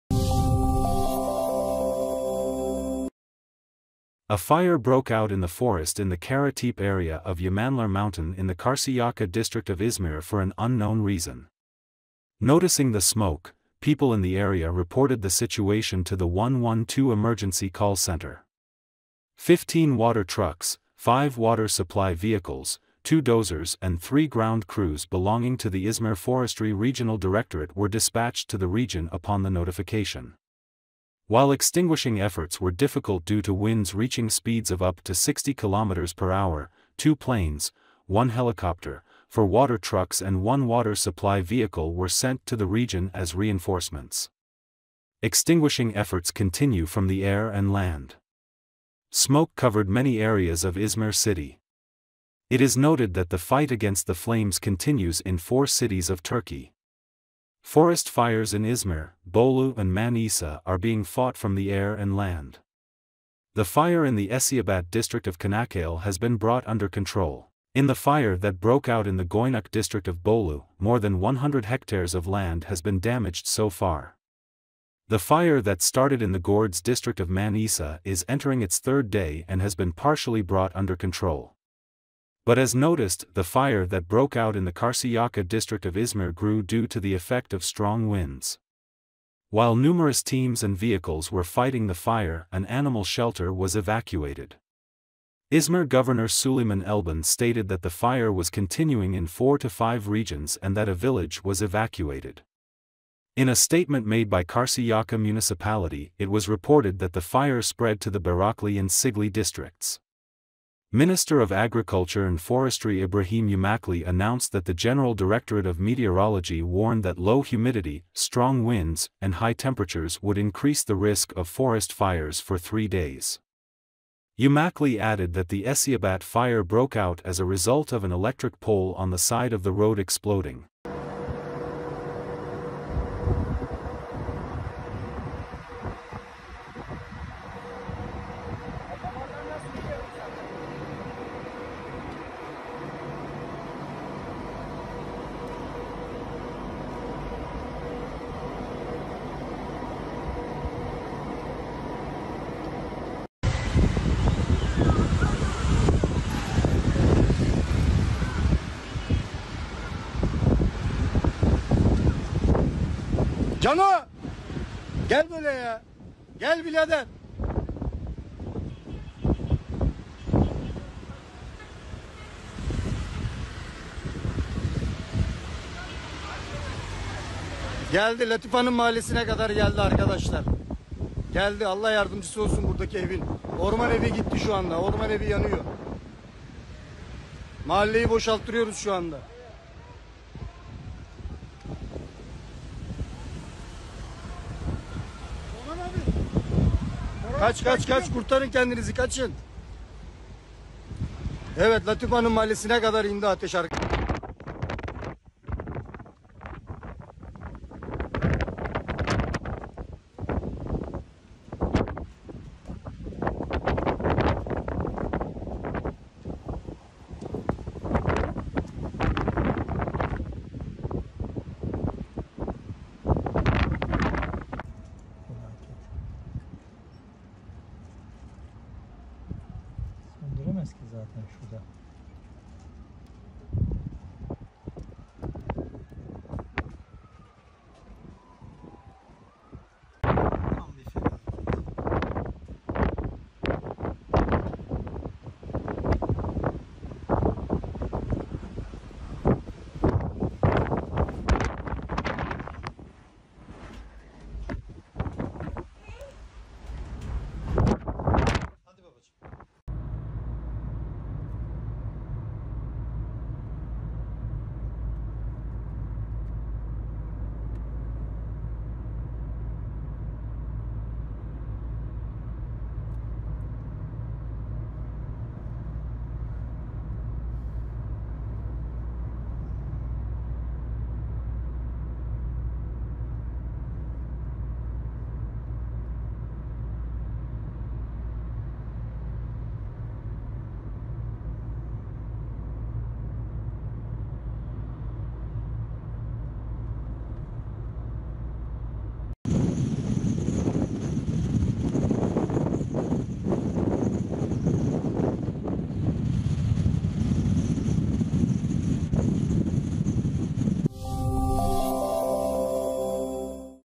Fire grows in Izmir of Turkey: The whole city is surrounded by smoke
A fire broke out in the forest in the Karatepe area of Yamanlar Mountain in the Karşıyaka district of İzmir  for an unknown reason. Noticing the smoke, people in the area reported the situation to the 112 Emergency Call Center. 15 water trucks, 5 water supply vehicles, 2 dozers and 3 ground crews belonging to the İzmir Forestry Regional Directorate were dispatched to the region upon the notification. While extinguishing efforts were difficult due to winds reaching speeds of up to 60 kilometers per hour, 2 planes, 1 helicopter, 4 water trucks and 1 water supply vehicle were sent to the region as reinforcements. Extinguishing efforts continue from the air and land. Smoke covered many areas of Izmir city.
İt is noted that the fight against the flames continues in 4 cities of Turkey. Forest fires in İzmir, Bolu and Manisa are being fought from the air and land. The fire in the Eceabat district of Çanakkale has been brought under control. In the fire that broke out in the Göynük district of Bolu, more than 100 hectares of land has been damaged so far. The fire that started in the Gördes district of Manisa is entering its 3rd day and has been partially brought under control. Bur as noticed, the fire that broke out in the Karşıyaka district of İzmir grew due to the effect of strong winds. While numerous teams and vehicles were fighting the fire, an animal shelter was evacuated. İzmir Governor Süleyman Elban stated that the fire was continuing in 4-5 regions and that a village was evacuated. In a statement made by the Karşıyaka Municipality, it was reported that the fire had spread to the Bayraklı and Çiğli districts. Minister of Agriculture and Forestry İbrahim Yumaklı announced that the General Directorate of Meteorology warned that low humidity, strong winds and high temperatures would increase the risk of forest fires for 3 days. Yumaklı added that the Eceabat fire broke out as a result of an electric pole on the side of the road exploding.